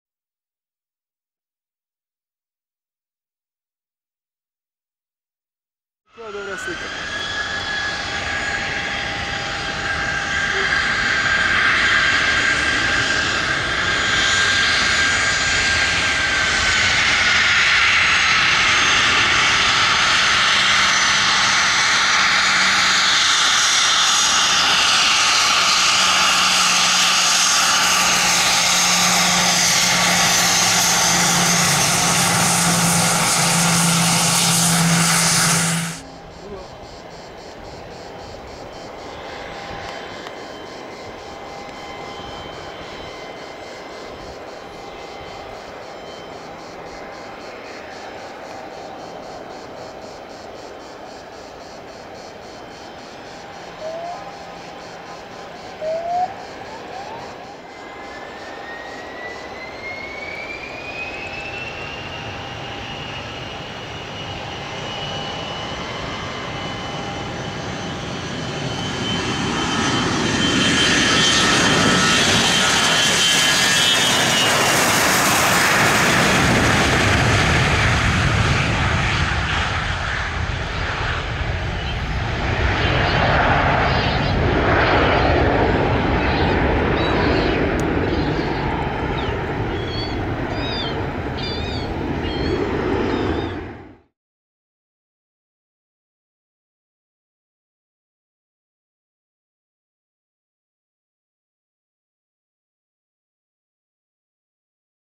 German Air Force Bombardier Global 5000 - Close-up Takeoff from Split Airport LDSP/SPU
Camera: Sony DSC-HX400V (Hand-held)
Aircraft: Bombardier Global 5000 (BD-700-1A11) (German Air Force)
Aircraft Registration: 14+04
Airport: SPU/LDSP Split Croatia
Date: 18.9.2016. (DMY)